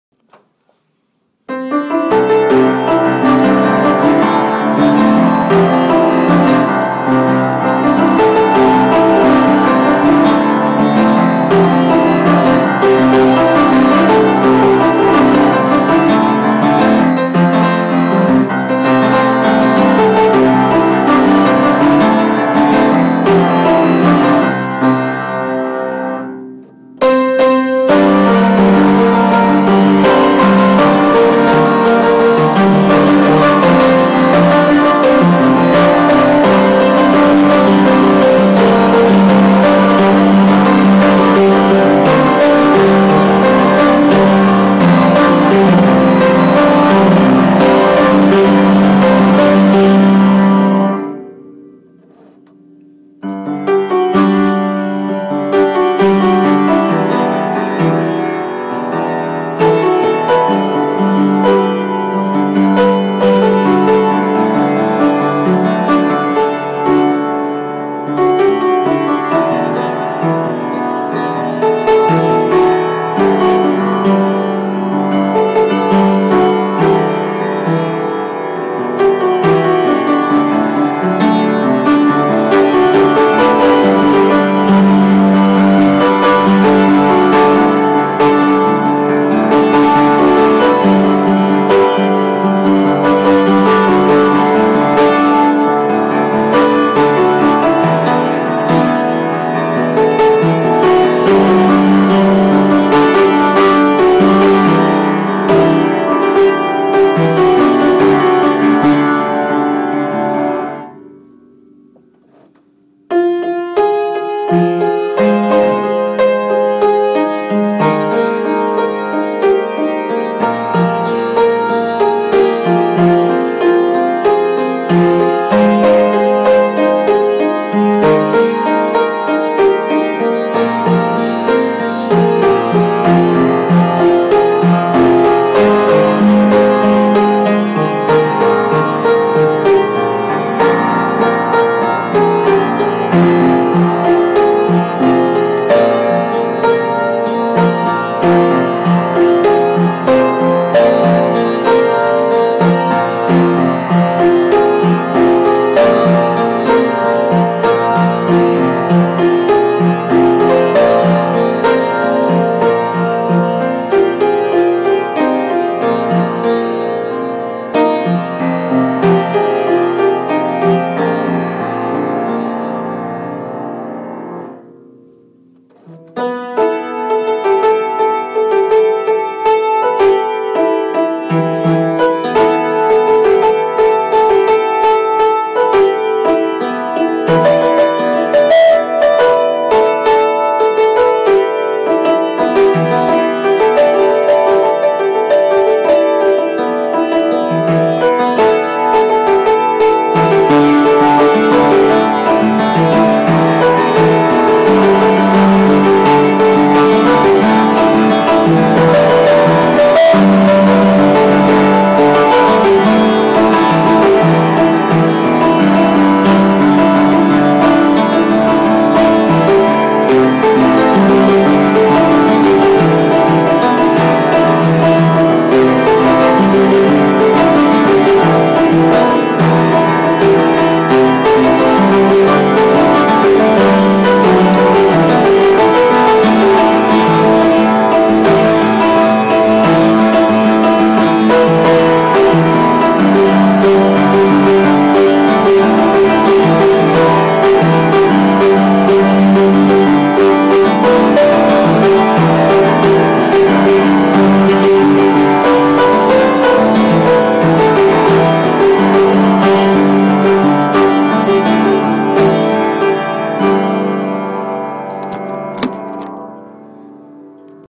Rhetoric 105 Presentation
Musical background for a speech for my final synthesis project for Rhetoric 105.